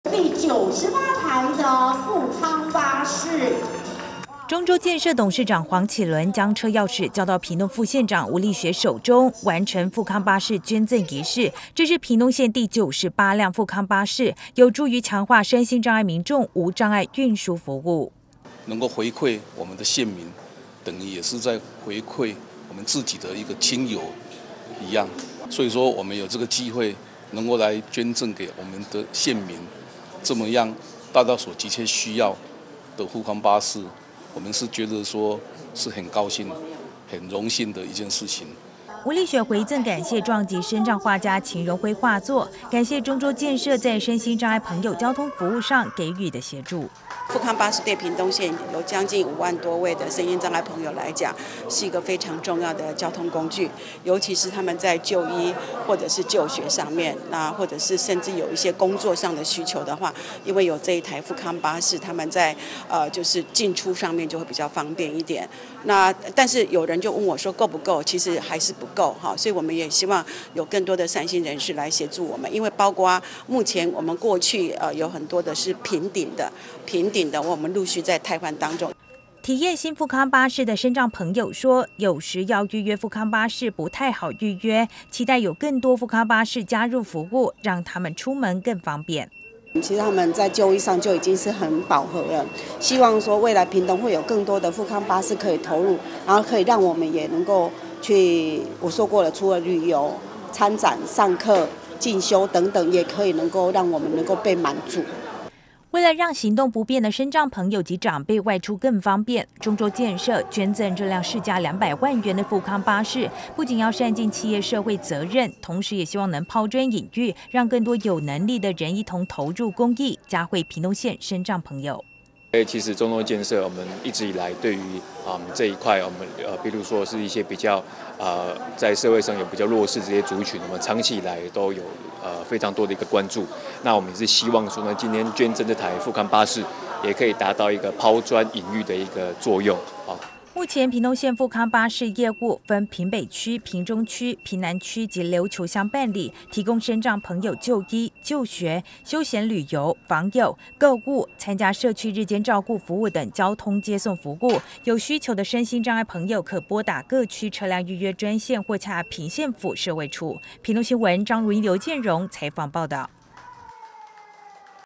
屏東市/社區關懷 110 0503 善盡企業社會責任 中洲建設捐贈復康巴士
復康巴士是身心障礙朋友最依賴的重要交通運具，屏東在地企業中洲建設捐贈屏東縣政府1輛價值2百萬元的復康巴士，提供身障朋友就醫、就學、休閒旅遊、訪友、購物、參加社區日間照顧服務等的交通接送服務，善盡企業社會責任。